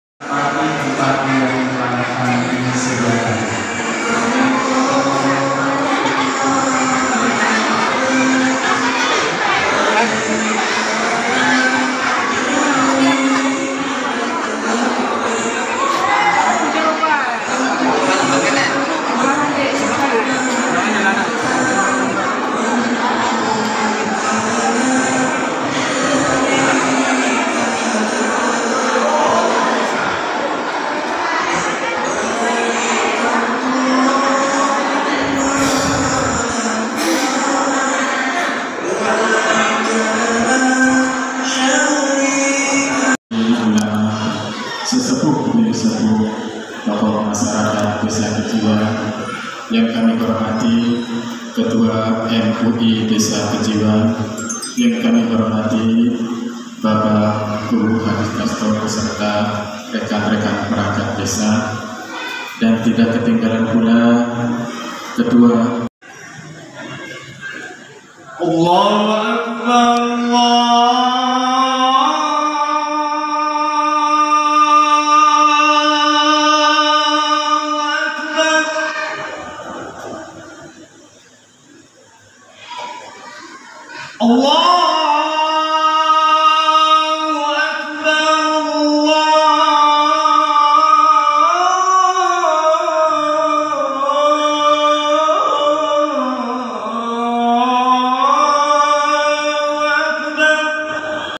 Semoga Menjadi Haji yang Mabrur dan Mabruroh
Calon Jamaah Haji Bpk.Suparno dan Ibu Darsem semoga menjadi Haji yang Mabrur dan Mabruroh, Amin.